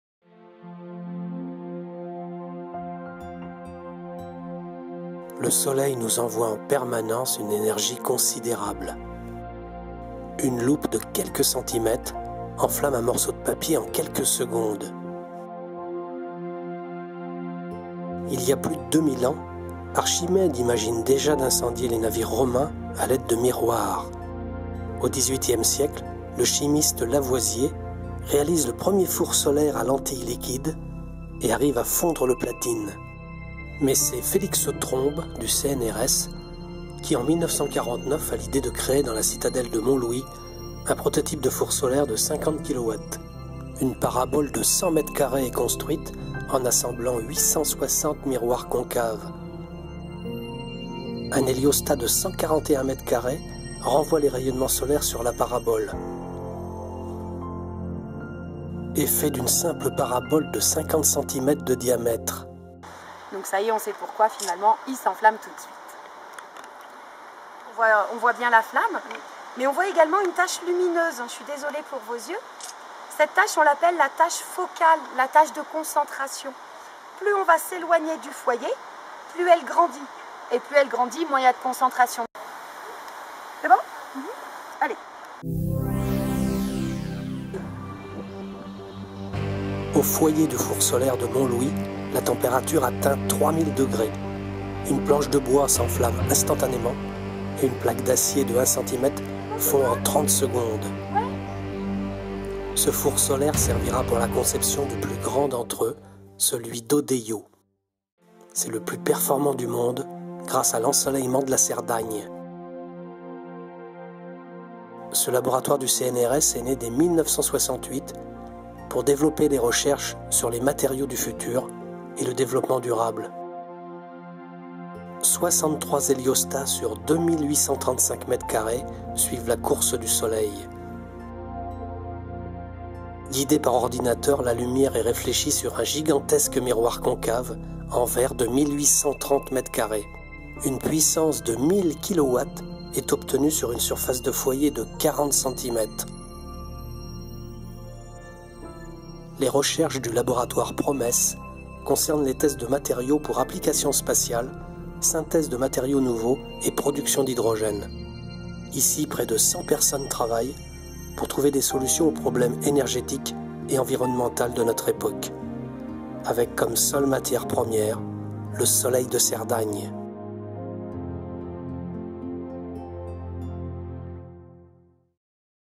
Fours solaires en Cerdagne : Mont-Louis et Odeillo
La Cerdagne ayant un des meilleurs ensoleillements de France, le premier four solaire de 50KW fut construit en 1947 dans la citadelle de Mont-Louis par l'équipe de Félix Trombe. Ce four servit  pour la construction du grand four solaire de 1000 KW d'Odeillo.
Les chercheurs du laboratoire Promes y mettent au point des procédés et matériaux nouveaux.
Cerdagne having one of the best hours of sunshine in south of France, the first 50KW solar oven was built in 1947 in the citadel of Mont-Louis by Félix Trombe's team. This furnace was used for the construction of the large solar furnace of 1000 KW of Odeillo.
Researchers from the Promes laboratory develop new processes and materials there.
prises de vues HD : panasonic DMC FZ38
montage : Studio Pinnacle version 14
english subtitles